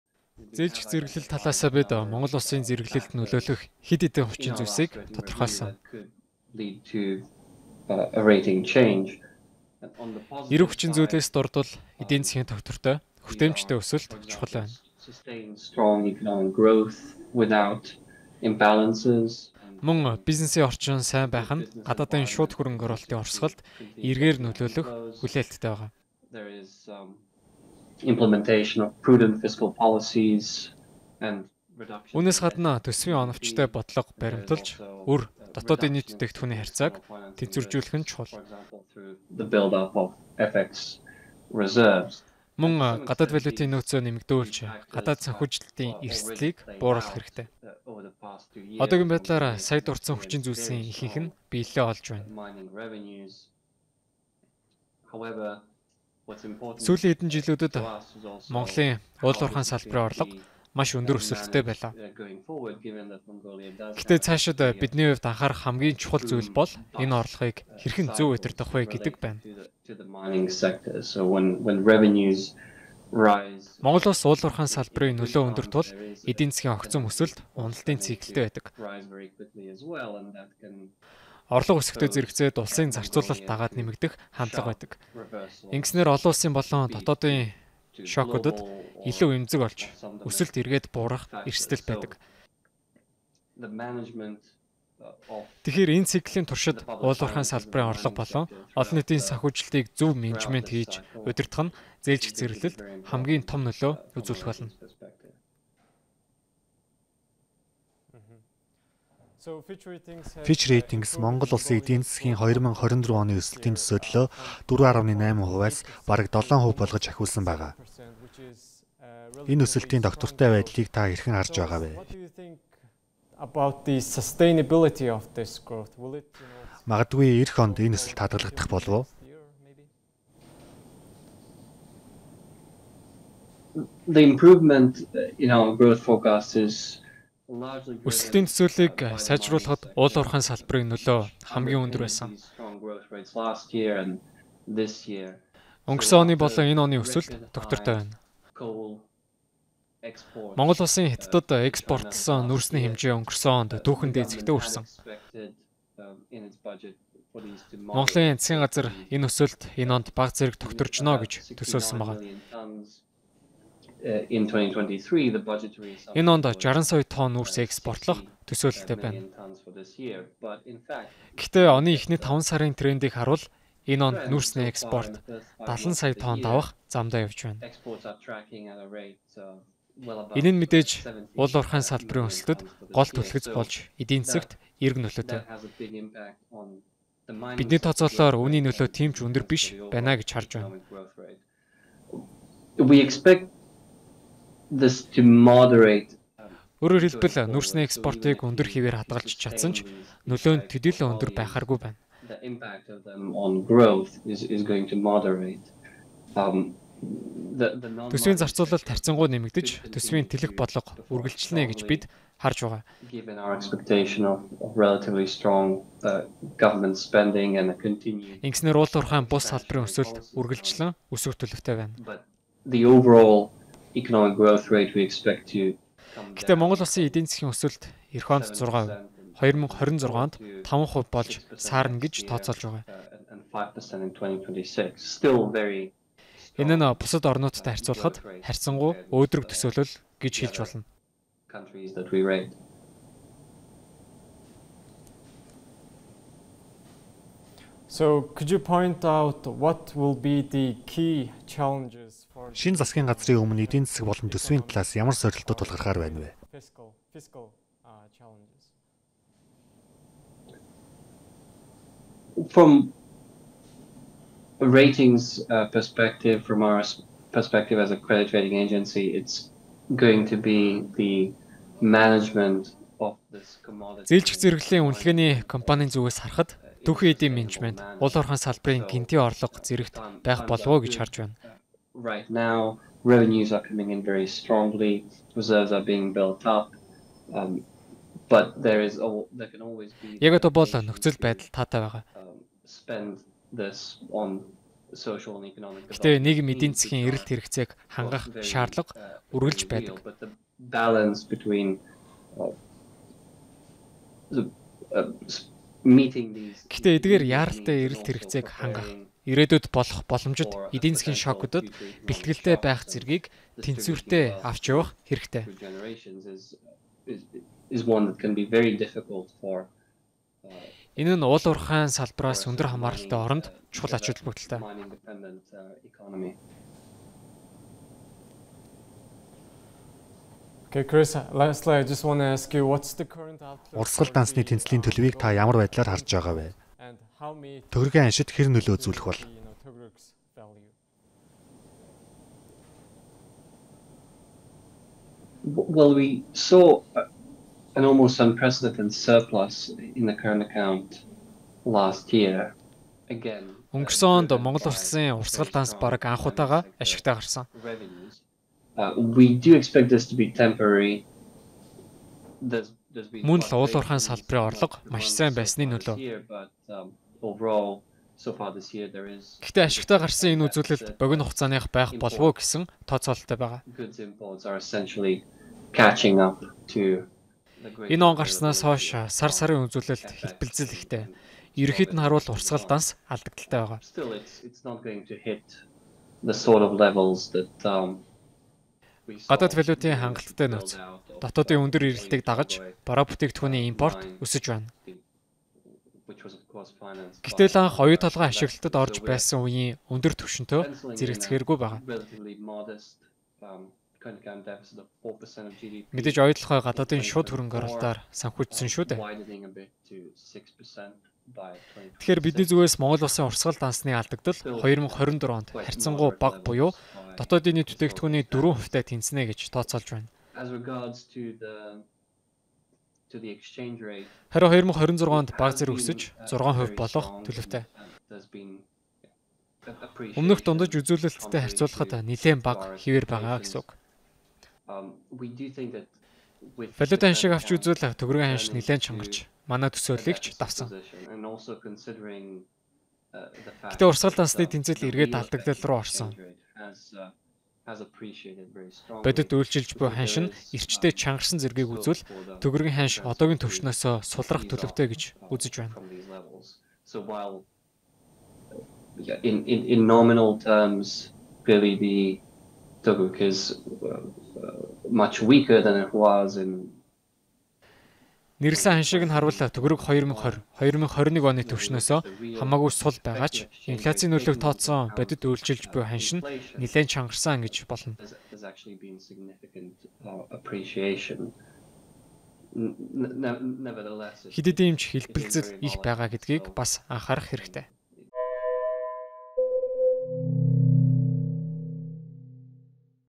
К.Крустинс: Төсвийн оновчтой бодлоготой байж өр, ДНБ-ий харьцааг тэнцвэржүүлэх нь чухал
Fitch Ratings-ийн шинжээч Крисжанис КрустинсДээрх нэвтрүүлэг нь Bloomberg TV Mongolia-ийн өмч бөгөөд хуулбарлан олшруулах, өөр платформ болон суваг руу хуулахыг хориглоно.